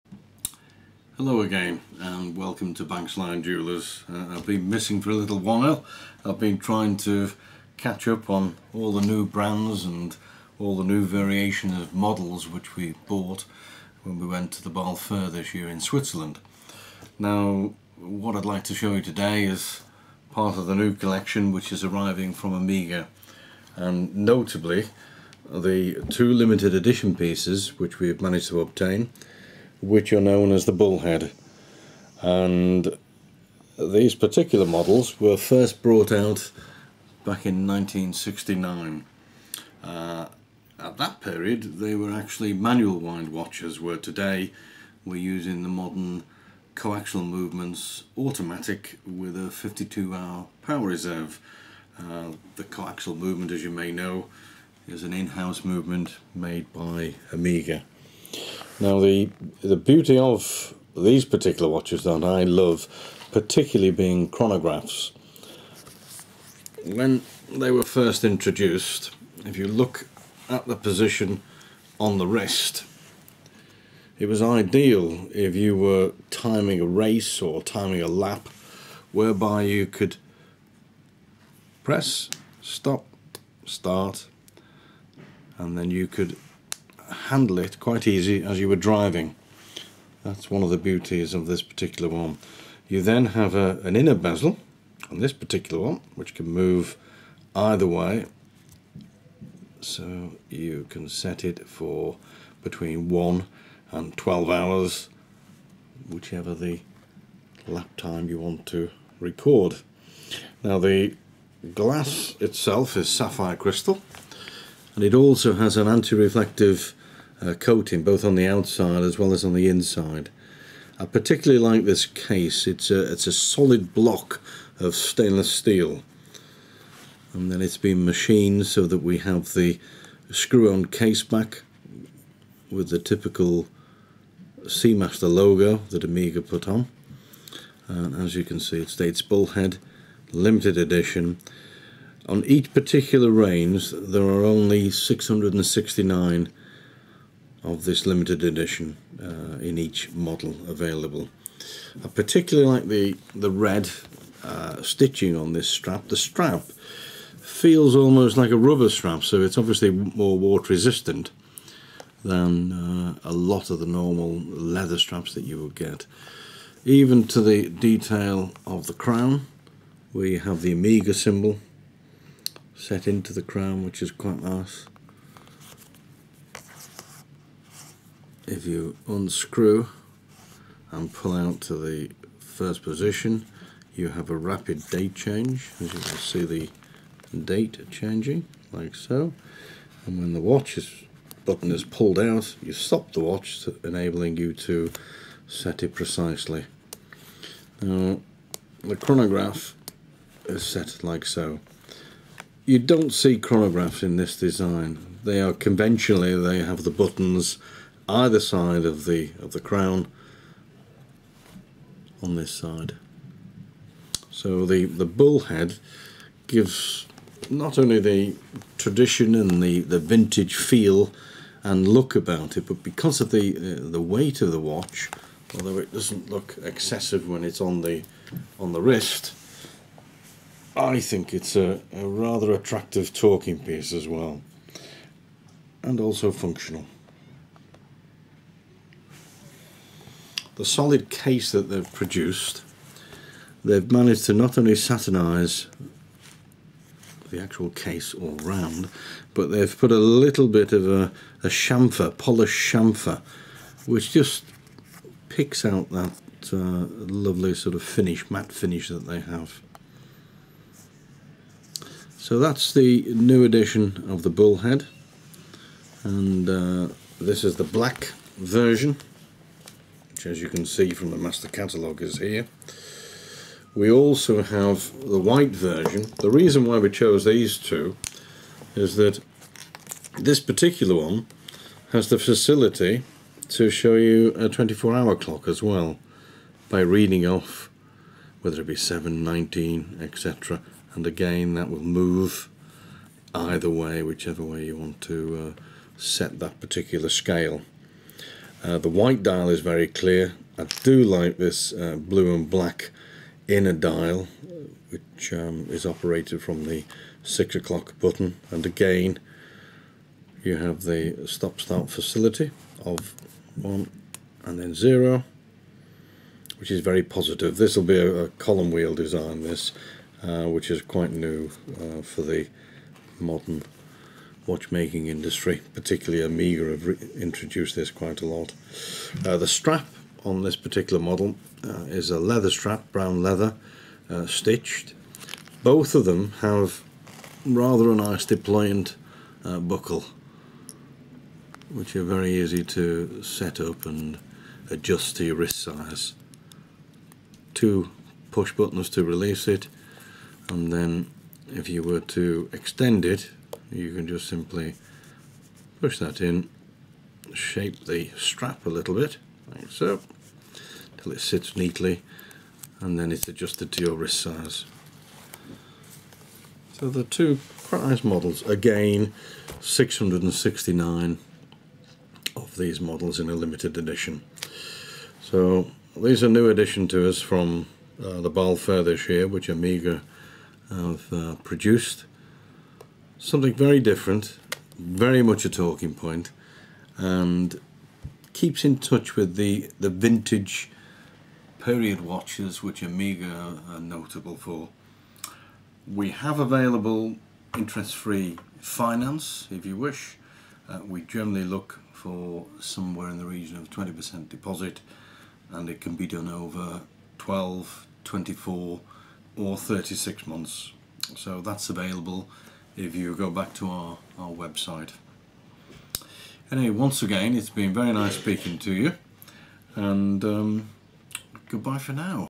Bob's Blog: The Omega Seamaster Bullhead Watches - 225.12.43.50.04.001 / 225.12.43.50.01.001
Bob's first video since visiting BASELWORLD 2014, in which he introduces two very different watches by OMEGA, the Limited Edition Seamaster Bullhead timepieces.

Banks Lyon Jewellers are an official Omega Stockist.